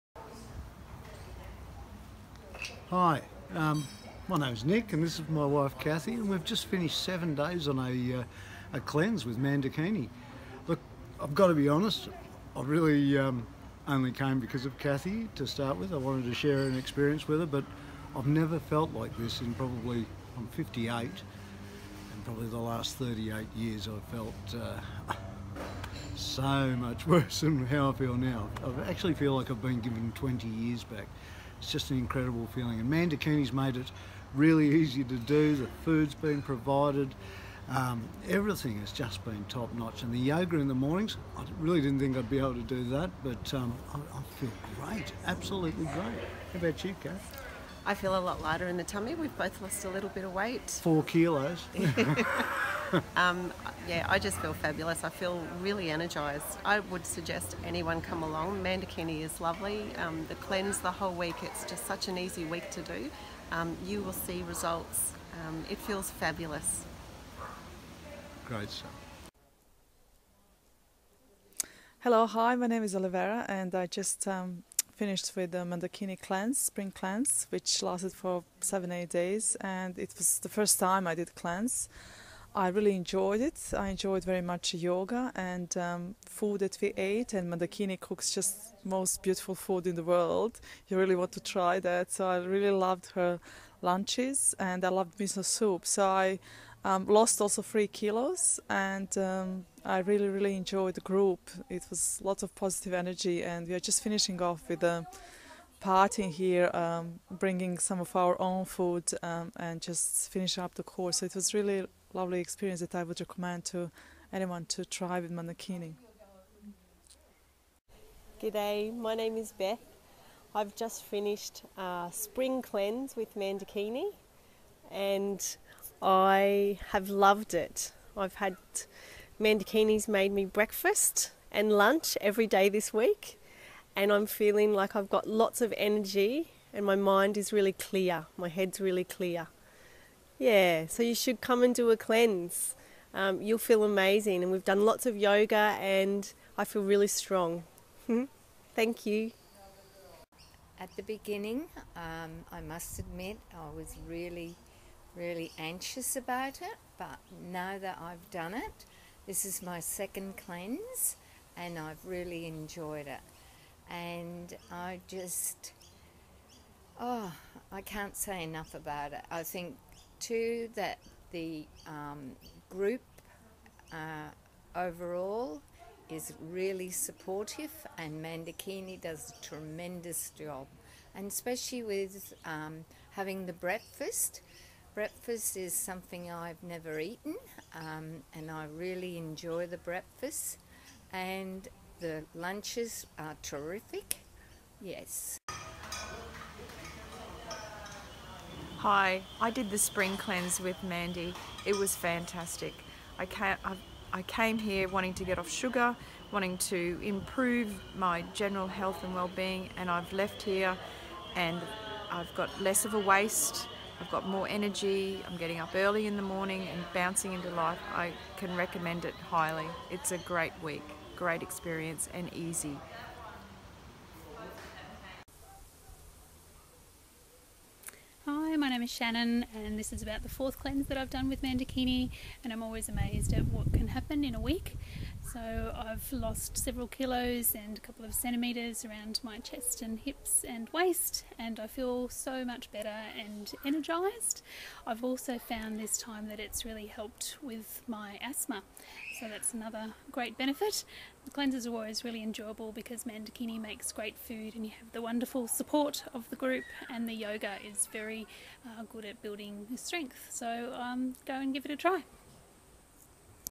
Cleanse's with Mandakini Testimonials
Some of our previous participants share the Benefits they have experienced during our Seasonal Cleanse/Detox programs